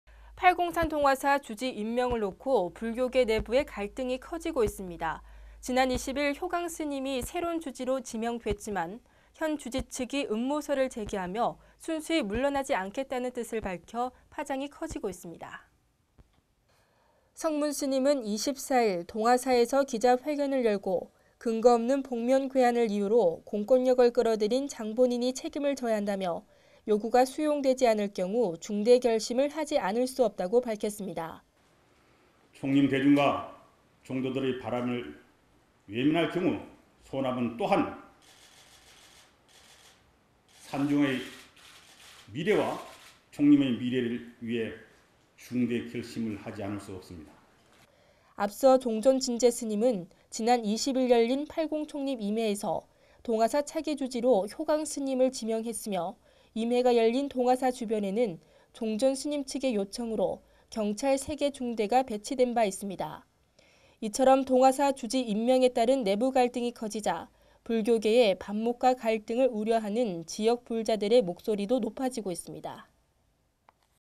동화사 사태 일으킨 장본인이 책임을
팔공산 동화사 주지 임명을 놓고 불교계 내부에 갈등이 커지고 있습니다. 지난 20일, 효광스님이 새로운 주지로 지명됐지만 현 주지 측이 음모설을 제기하며 순수히 물러나지 않겠다는 뜻을 밝혀 파장이 커지고 있습니다.